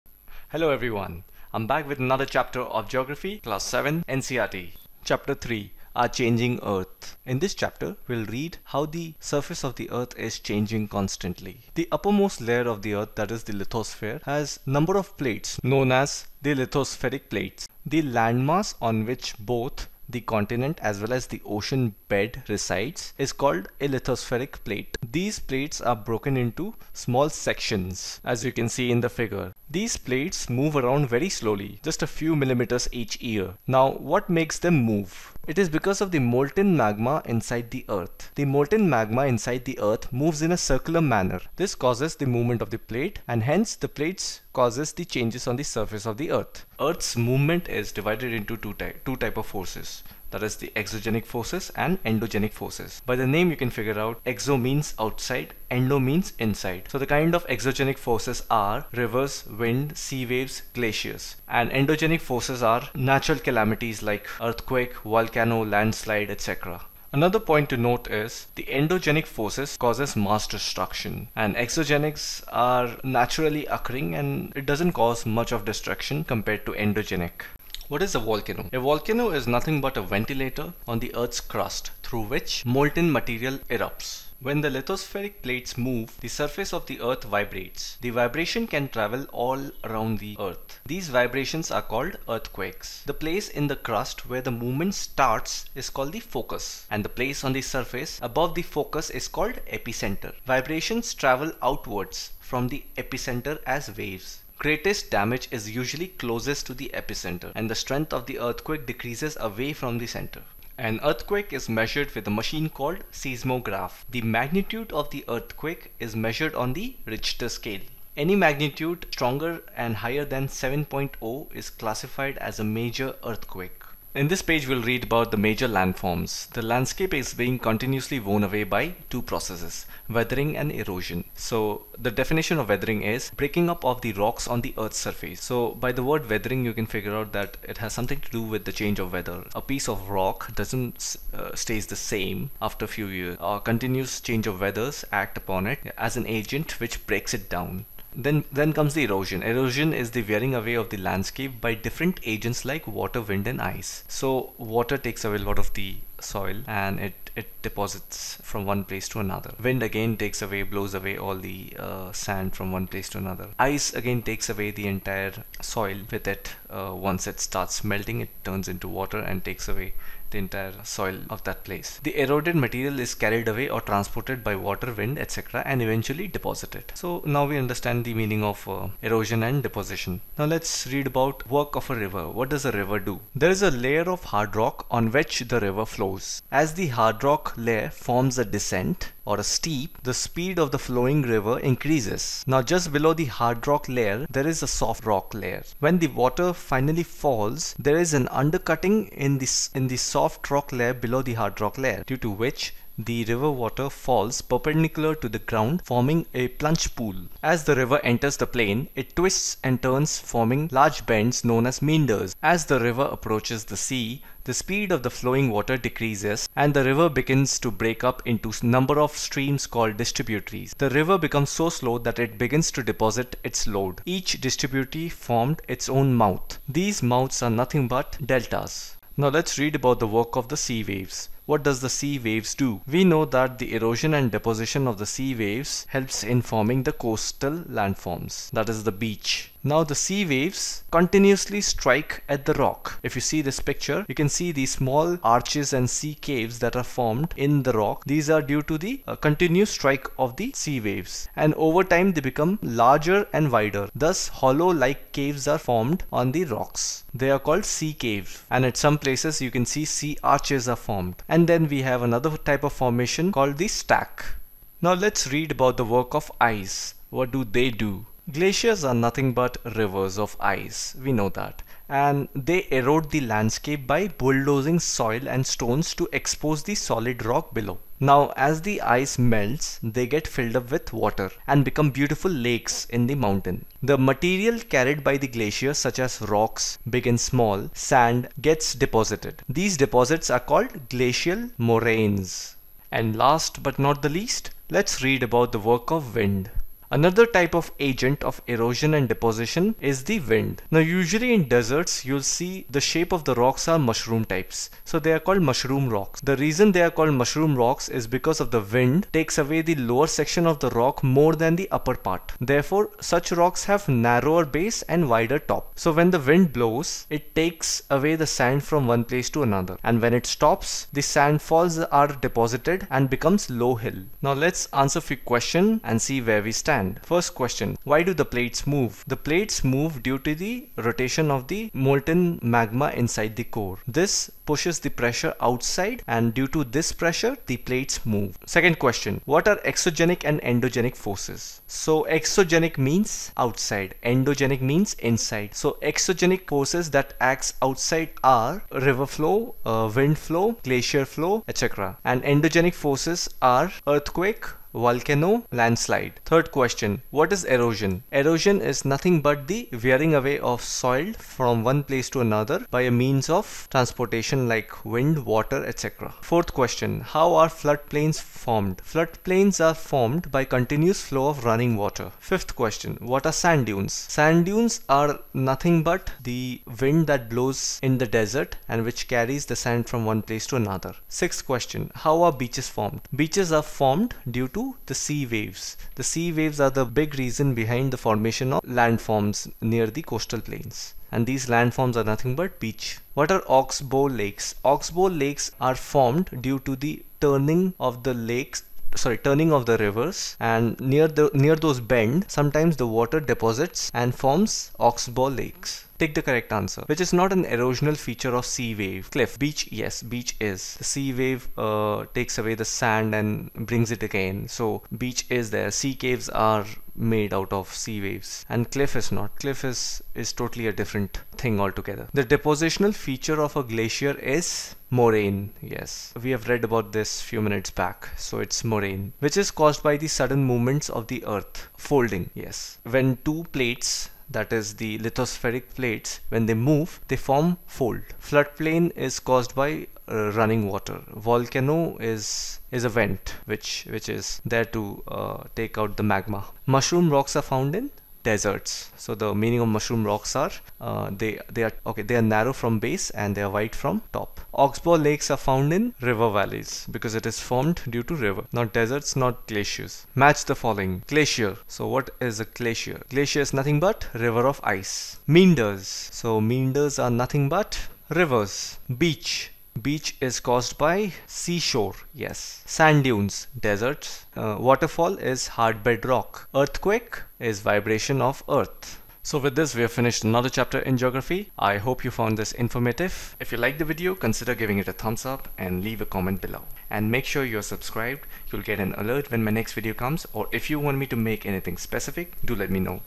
Our Changing Earth - Chapter 3 Geography NCERT class 7
Whether you are preparing for CSAT, CDS, UPSC, PO, CAPF, IB, CGL or anything. This is a small video snippet that covers chapter 3 "Our Changing Earth" of class 7 Geography NCERT book. You can watch this video without having to read the entire book full of texts. Happy studying!

--- You can also view playlists of other NCERT Geography videos:-